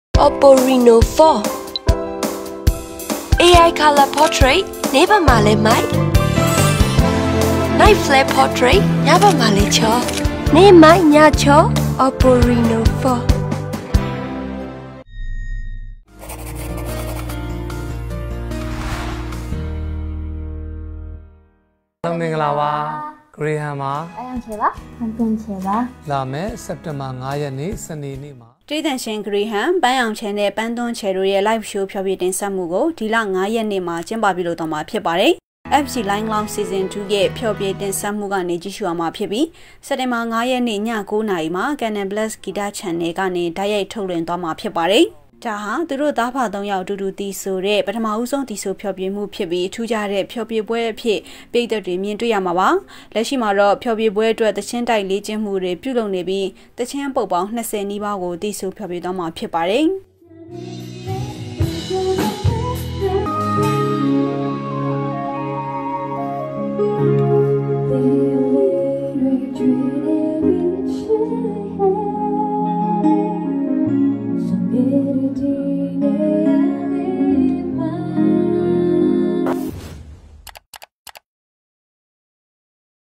ဂရေဟမ်တို့သားအဖသုံးယောက်ရဲ့ Live Show
စက်တင်ဘာ - ၂၊၂၀၂၀

တေးသံရှင် ဂရေဟမ်၊ ပန်းရောင်ခြယ်နဲ့ ပန်းသွန်းခြယ်တို့ရဲ့ Live Show ဖျော်ဖြေတင်ဆက်မှုကို ဒီလ ၅ ရက် မှာ ပြုလုပ်ကျင်းပသွားမှာဖြစ်ပါတယ်။

FG Live Lounge Season 2 ရဲ့ ဖျော်ဖြေတင်ဆက်မှုကနေ ကြည့်ရှုရမှာဖြစ်ပြီး စက်တင်ဘာ ၅ ရက်နေ့ ညကိုးနာရီမှာ Canal + Gita Channel ကနေ တိုက်ရိုက်ထုတ်လွှင့်သွားမှာပါ။

ဒါဟာ သူတို့သားအဖသုံးယောက်အတူတူသီဆိုတဲ့ ပထမဆုံးသီဆိုဖျော်ဖြေမှုဖြစ်ပြီး ထူးခြားတဲ့ဖျော်ဖြေပွဲတစ်ခုအဖြစ် ပရိသတ်တွေမြင်တွေ့ရမှာပါ။

လက်ရှိမှာတော့ ဖျော်ဖြေပွဲအတွက် သီချင်းတိုက်လေ့ကျင့်မှုတွေကို ပြုလုပ်နေပြီး သီချင်းအပုဒ်ပေါင်း ၂၀ နီးပါး သီဆိုဖျော်ဖြေသွားမှာဖြစ်ပါတယ်။

သတင်း - အေးမြတ်